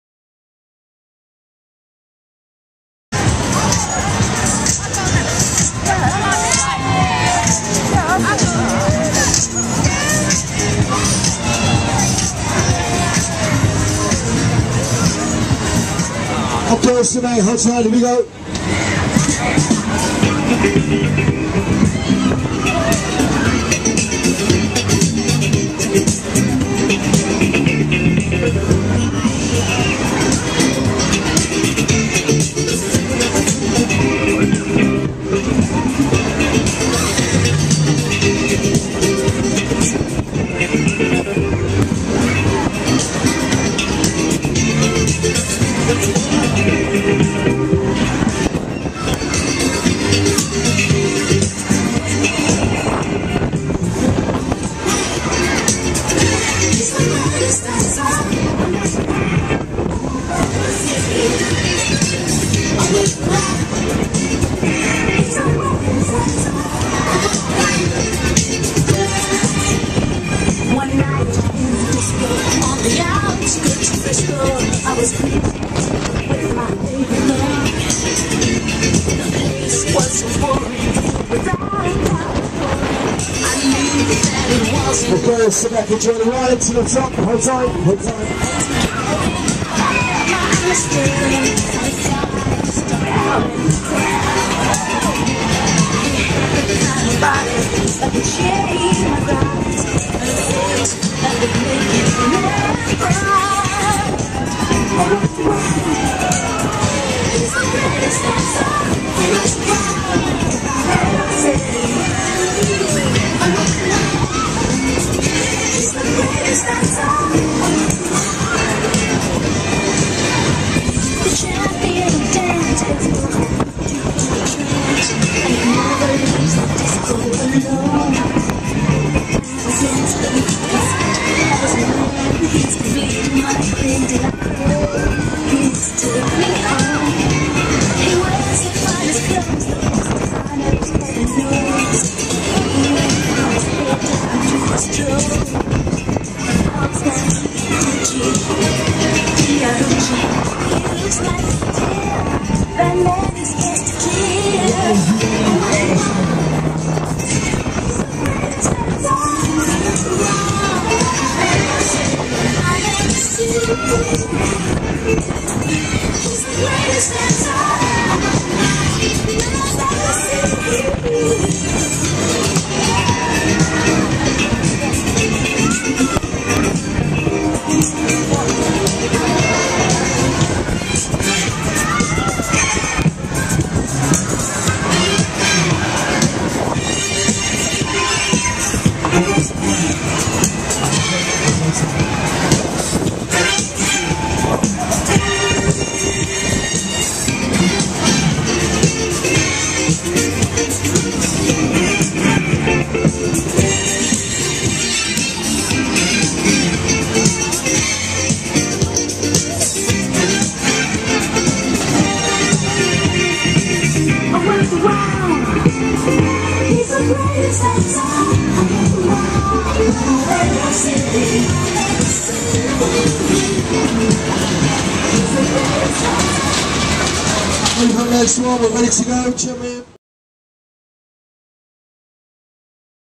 Sky Diver Onride Wimbledon Common Funfair
Onride of Albert & Raymond Boyd's 'Sky Diver' Lifting Paratrooper at Wimbledon Common Funfair, June 2013